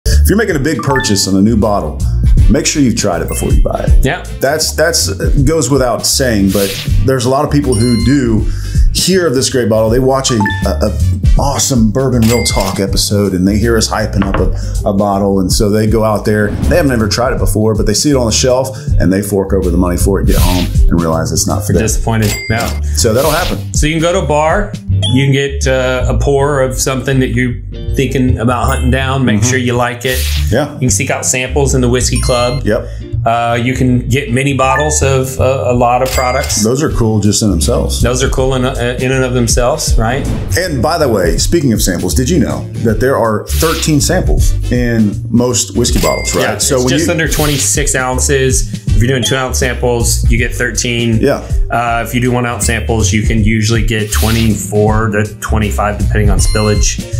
Why it is Important to Try Out a Whiskey Before Making a Big Purchase (Short)
Making a big whiskey purchase? Try before you buy!

Don't be fooled by marketing hype. Seek out samples at a bar or whiskey club to ensure you'll enjoy it.